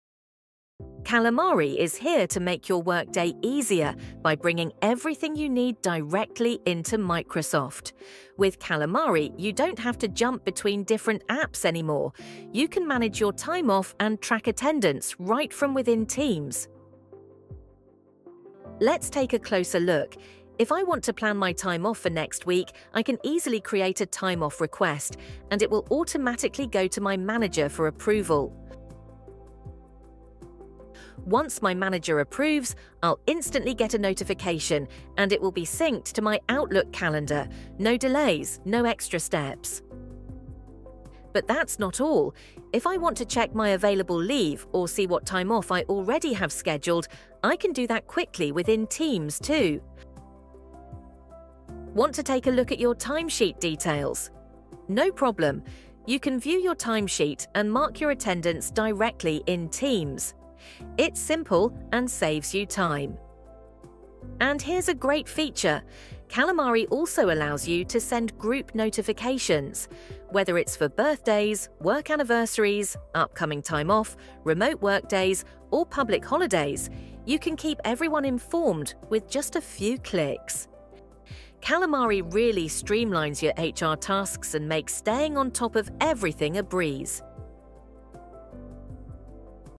Calamari & Microsoft 365: HR Software in MS Teams for time-off & attendance management
Calamari brings powerful yet simple HR management directly into your Microsoft 365 environment. Eliminate app-switching and boost productivity with seamless time off management and attendance tracking in Microsoft Teams nad Outlook.
✓ SIMPLIFY EMPLOYEE TIME OFF: Create, approve, and track leave requests that sync automatically with Outlook calendars
✓ ENHANCE TEAM COORDINATION: Send automated group notifications about absences, work anniversaries, and public holidays
✓ MAXIMIZE PRODUCTIVITY: Access leave balances and timesheets directly in Microsoft Teams
✓ REDUCE ADMINISTRATIVE BURDEN: Eliminate manual HR processes with intuitive interfaces designed for busy professionals

Don't have Calamari yet? Sign up for a test account and check out the app's capabilities.